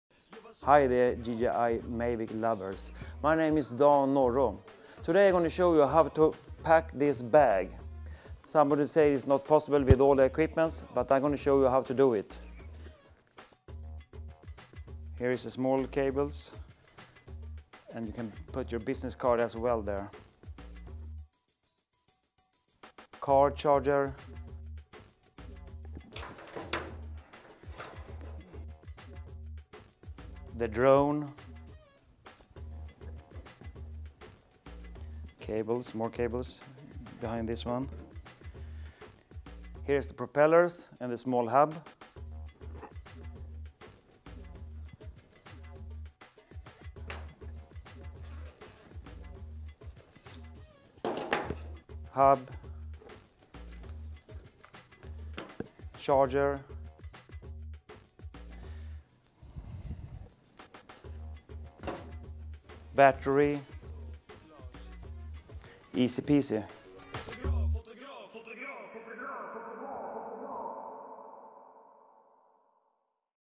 DJI Mavic Pro How to pack your Mavic's bag
A quick brief  in 70 seconds how to pack your bag and how I prefer to stow my equipment in DJI Mavic bag.
Follow me on instagran @dannesweden